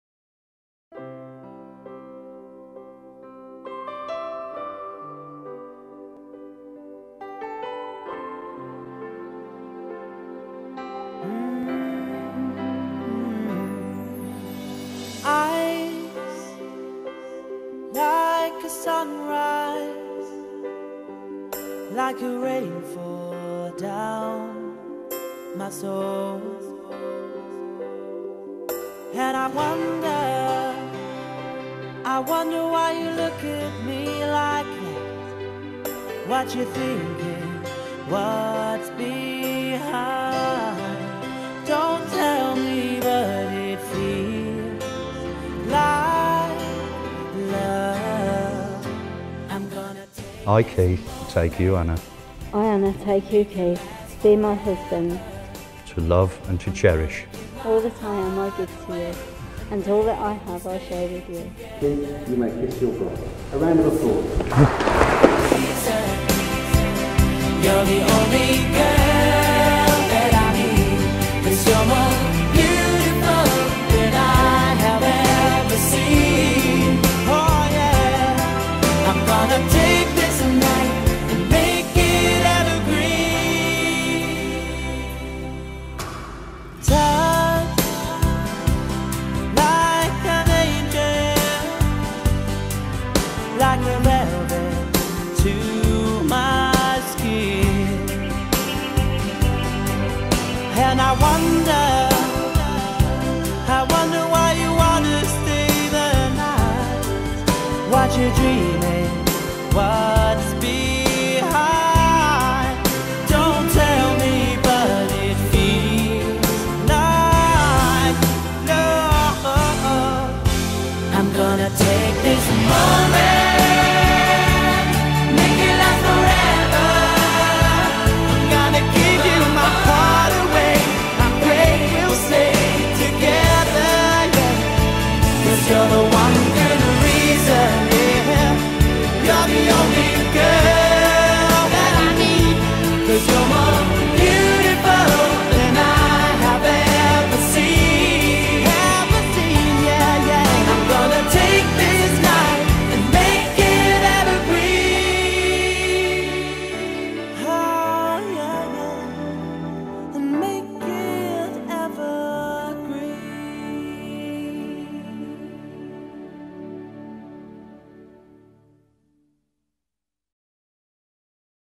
Anna & Keith FISHLEY PARK Wedding Highlights
Fun, love and laughter at this uber friendly wedding with lots of colour. for Anna and Keith at St Mark's Church, Great Wyrley and Fishley Park, Wolverhampton